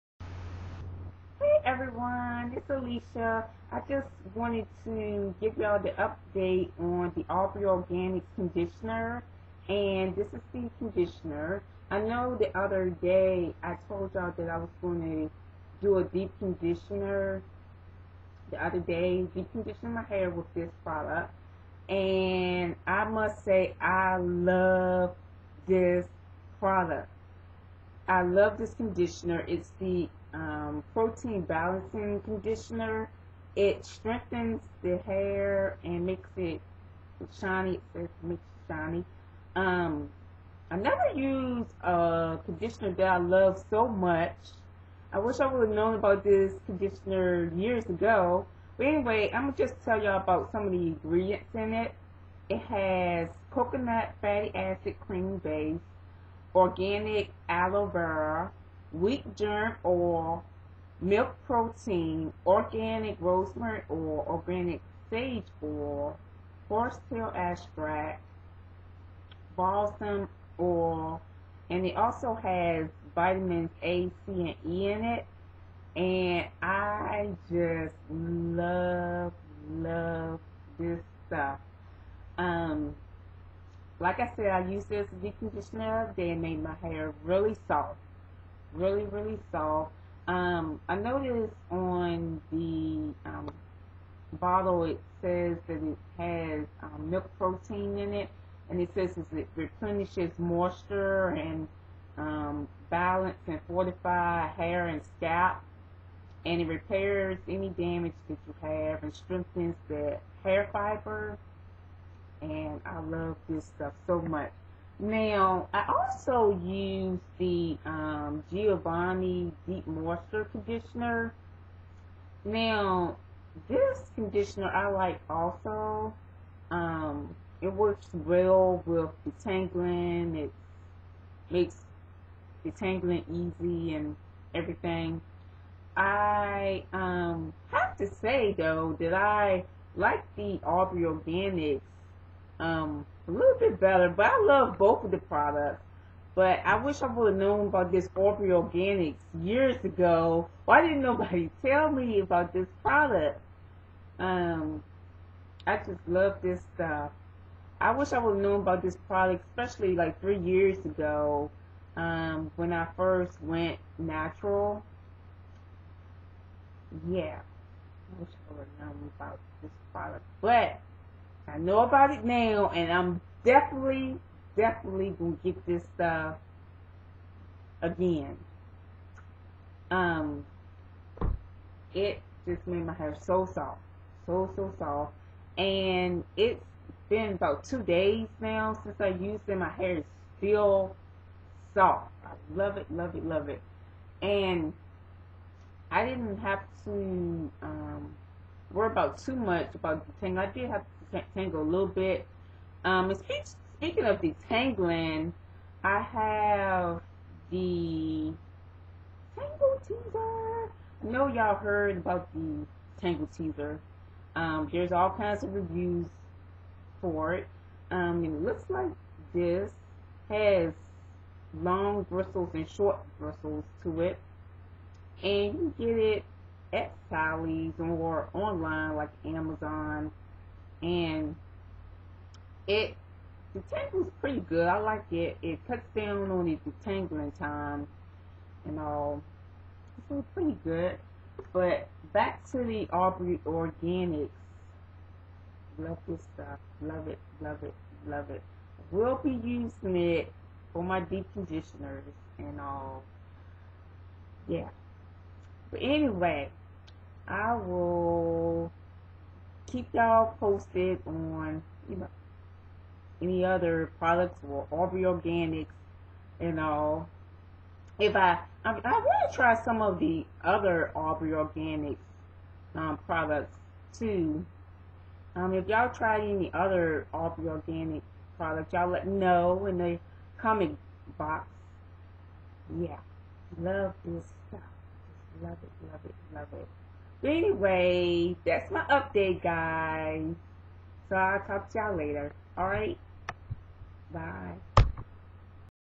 Deep Condition: Aubrey Organics Glycogen Protein Balancing Conditioner Review/ Natural Hair
My thoughts on Glycogen Protein Balancing Conditioner. Also my thoughts on the tangle teezer.